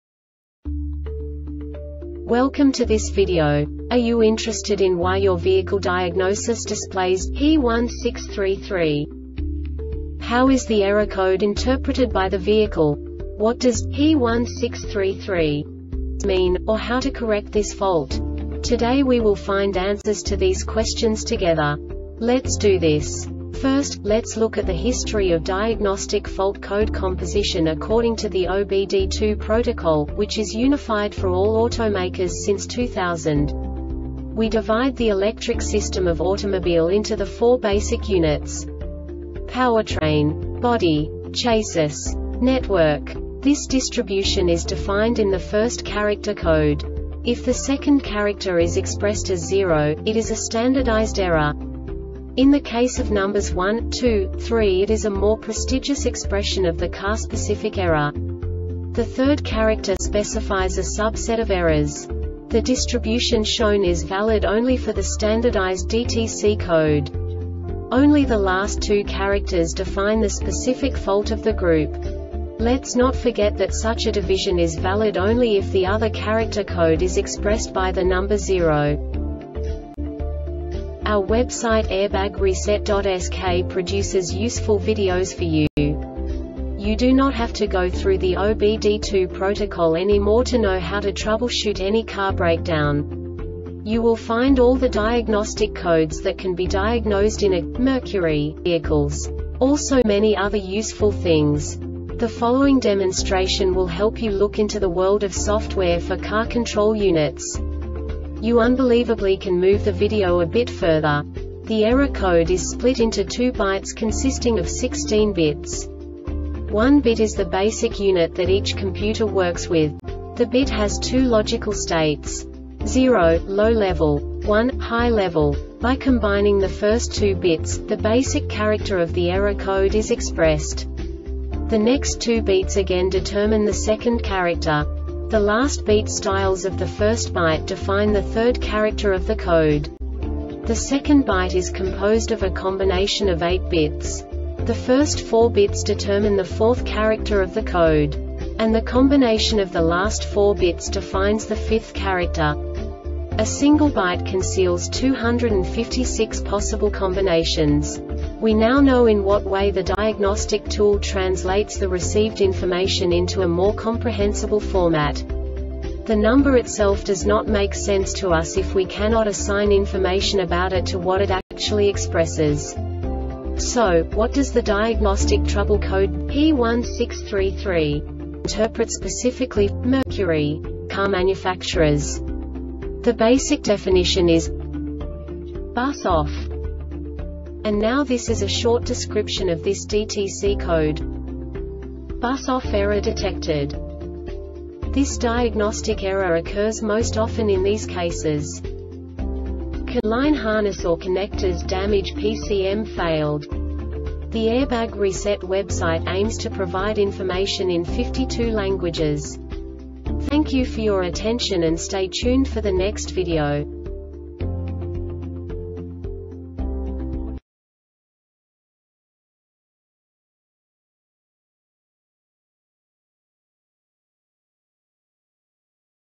DTC Mercury P1633 Short Explanation
Contents:
0:21 Basic DTC analysis according to OBD2 protocol standard.
1:48 Insight into programming
2:58 A diagnostically understandable description of the Diagnostic Trouble Code
3:05 Basic error definition

Make
Mercury
Code
P1633
Definition
Bus Off
Description
Bus off error detected
Cause
CAN line harness or connectors damagePCM failed